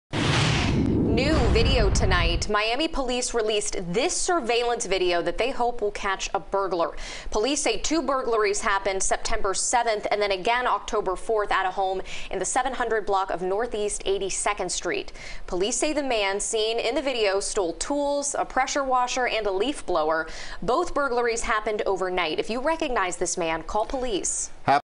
Miami Police Looking For Man They Believed Burglarized Same Home Twice
Miami Police released a video Friday in hopes to capture a man they believe burglarized a Miami residence twice and stole maintenance tools.